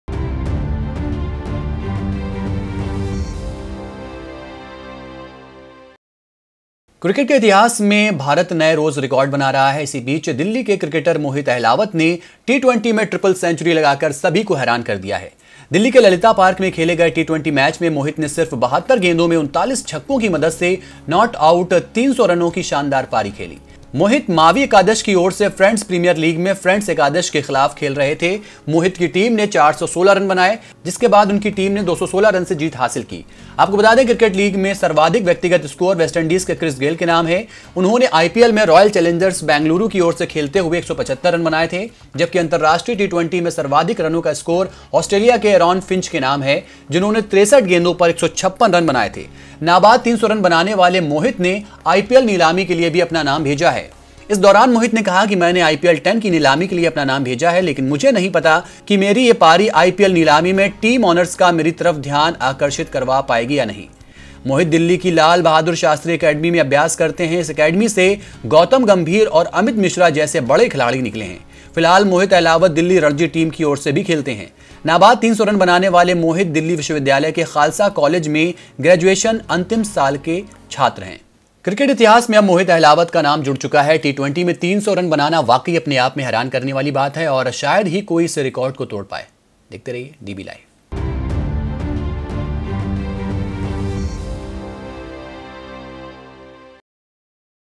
DB LIVE | 8 FEB 2017 | Delhi boy who hit 300 in a T20 match Mohit Ahlawat
क्रिकेट इतिहास में भारत रोज नए रिकॉर्ड बना रहा है। इसी बीच दिल्ली के क्रिकेटर मोहित अहलावत ने टी-20  में ट्रिपल सेंचुरी लगाकर सभी को हैरान कर दिया है। दिल्ली के ललिता पार्क में खेले गए टी-20  मैच में मोहित ने सिर्फ 72  गेंदों में 39  छक्कों की मदद से नॉट आउट 300  रनों की शानदार पारी खेली। मोहित मावी एकादश की ओर से फ्रेंड्स प्रीमियर लीग में फ्रेंड्स एकादश के खिलाफ खेल रहे थे। मोहित की टीम ने 416 रन बनाए, जिसके बाद उनकी टीम ने 216 रन से जीत हांसिल की। आपको बता दे कि क्रिकेट लीग में सर्वाधिक व्यक्तिगत स्कोर वेस्टइंडीज के क्रिस गेल के नाम हैं। उन्होंने आईपीएल में रॉयल चैलेंजर्स बैंगलोर की ओर से 175 रन बनाए थे। जबकि अंतरराष्ट्रीय टी-20 में सर्वाधिक रनों का स्कोर ऑस्ट्रेलिया के एरॉन फिंच के नाम है जिन्होंने 63 गेंदों पर 156 रन बनाए थे। नाबाद 300 रन बनाने वाले मोहित ने आईपीएल नीलामी के लिए भी अपना नाम भेजा है। इस दौरान मोहित ने कहा कि मैंने आईपीएल-10 की नीलामी के लिए अपना नाम भेजा है लेकिन मुझे नहीं पता कि मेरी ये पारी आईपीएल नीलामी में टीम ऑनर्स का मेरी तरफ ध्यान आकर्षित करवा पाएगी या नहीं। मोहित दिल्ली की लाल बहादुर शास्त्री एकेडमी में अभ्यास करते हैं, इस एकेडमी से गौतम गंभीर और अमित मिश्रा जैसे बड़े खिलाड़ी निकले हैं। फिलहाल मोहित अहलावत दिल्ली रणजी टीम की ओर से भी खेलते हैं। नाबाद 300 रन बनाने वाले मोहित दिल्ली विश्वविद्यालय के खालसा कॉलेज में ग्रैजुएशन अंतिम साल के छात्र हैं।
क्रिकेट इतिहास में अब मोहित अहलावत का नाम जुड़ चुका है। टी-20 में 300 रन बनाना वाकई अपने आप में हैरान करने वाला है औऱ शायद ही कोई इस रिकॉर्ड को तोड़ सके. देखते रहिए डीबी लाइव.